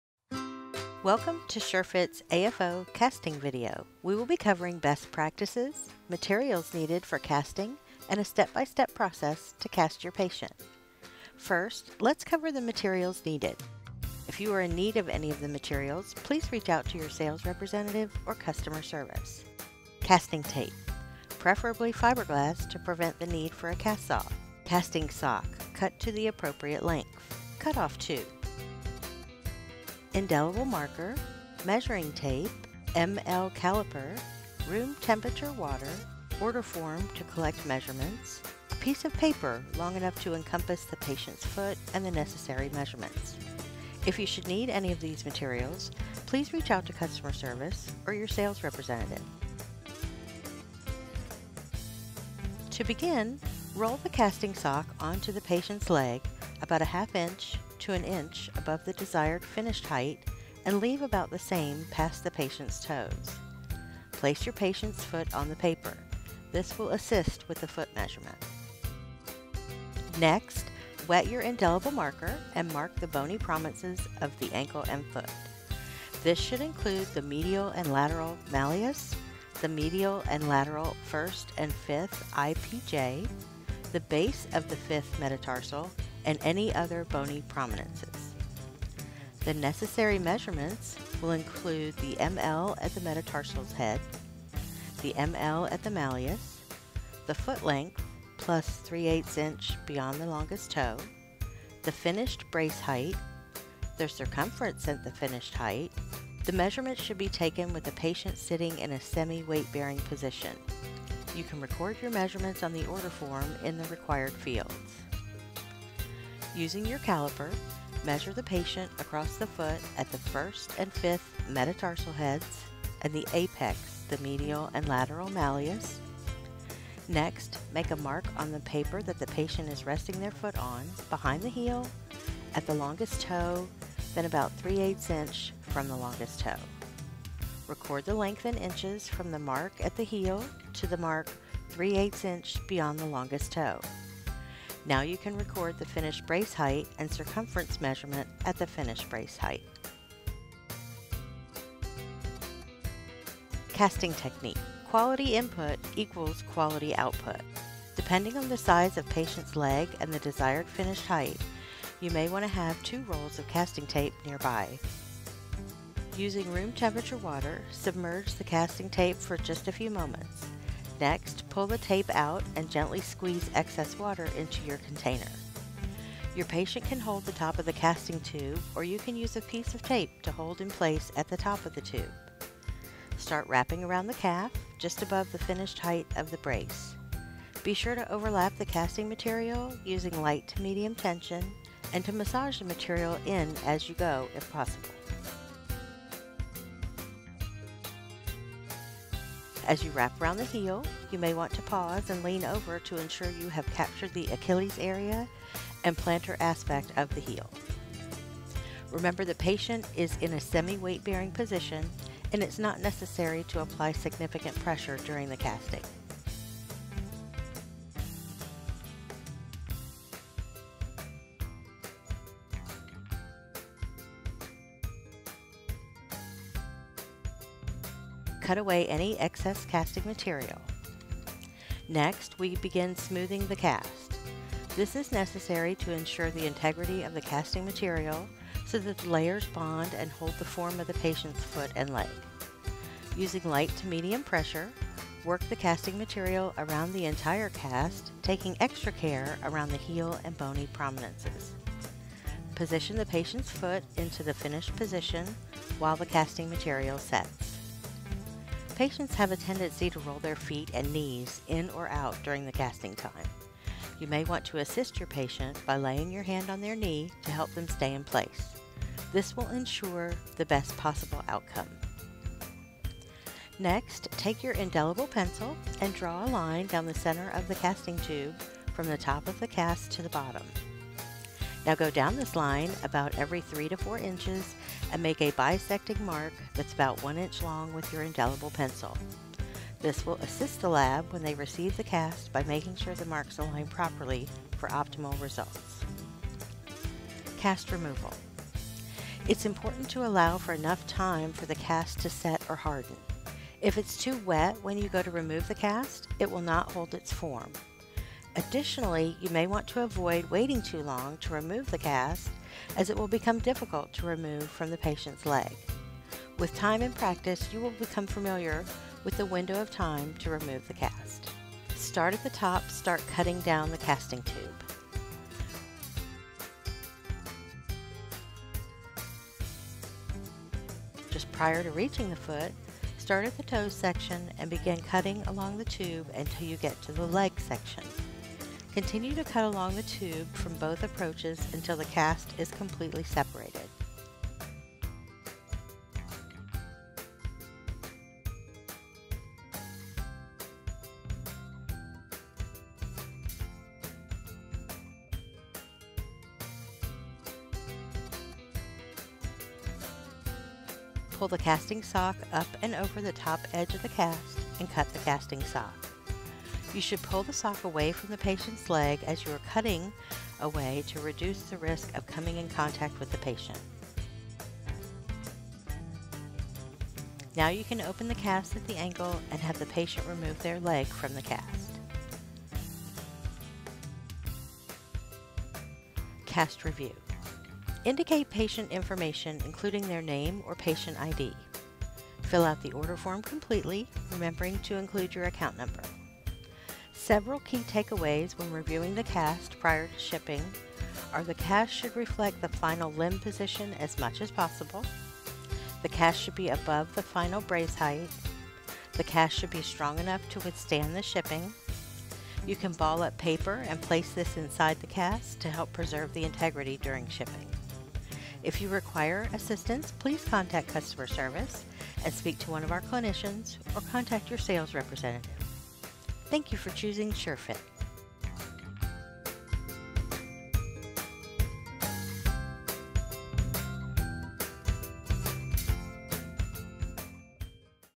How to Cast a Custom AFO
In this video, we cover the materials needed, a step-by-step process, and best practices for casting a custom AFO.